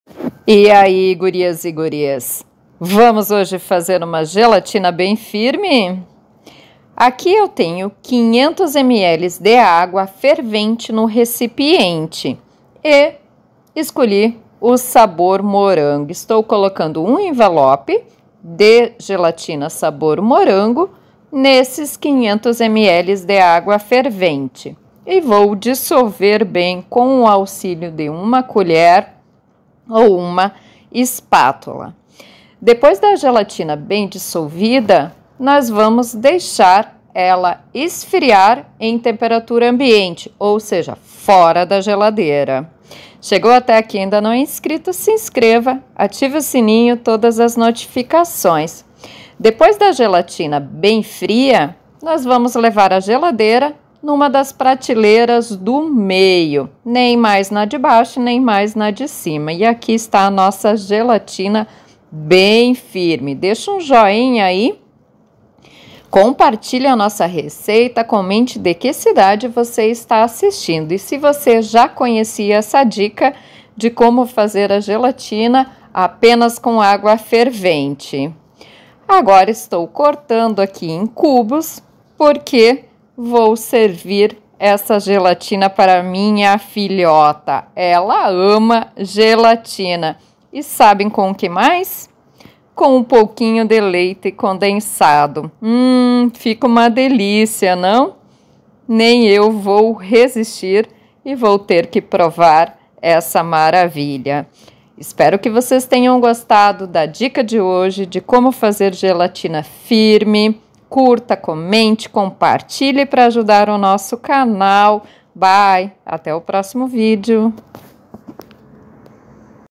COMO FAZER GELATINA MAIS FIRME?
COMO FAZER GELATINA FIRME? | Como fazer gelatina

Ingredientes
1 gelatina do sabor de preferência
500 ml de água quente

Facebook: Receitas e Dicas da Sol